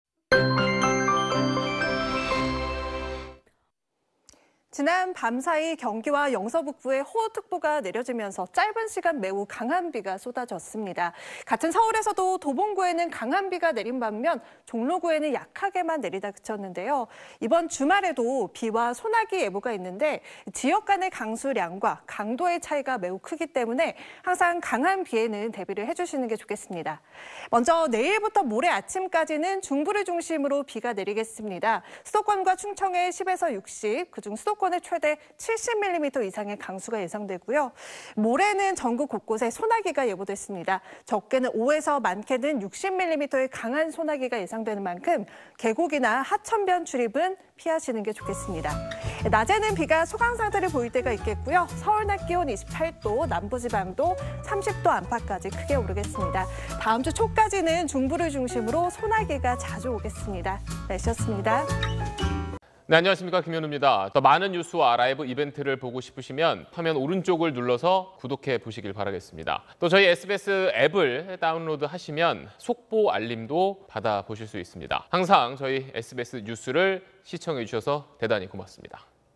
[날씨] 주말, 비·소나기 예보…지역 간 강수 편차 커 / SBS 8뉴스
지난 밤사이 경기와 영서 북부에 호우 특보가 내려지면서 짧은 시간 매우 강한 비가 쏟아졌습니다.

같은 서울에서도 도봉구에는 강한 비가 내린 반면 종로구에는 약하게만 내리다 그쳤는데요.

이번 주말에도 비와 소나기 예보가 있는데, 지역 간의 강수량과 강도의 차이가 매우 크기 때문에 항상 강한 비에는 대비를 해 주시는 게 좋겠습니다.

먼저 내일(10일)부터 모레 아침까지는 중부를 중심으로 비가 내리겠습니다.

수도권과 충청에 10~60, 그중 수도권에 최대 70mm 이상의 강수가 예상되고요, 모레는 전국 곳곳에 소나기가 예보됐습니다.

적게는 5에서 많게는 60mm의 강한 소나기가 예상되는 만큼 계곡이나 하천변 출입은 피하시는 게 좋겠습니다.

낮에는 비가 소강상태를 보일 때가 있겠고요, 서울 낮 기온 28도, 남부지방도 30도 안팎까지 크게 오르겠습니다.

다음 주 초까지는 중부를 중심으로 소나기가 자주 오겠습니다.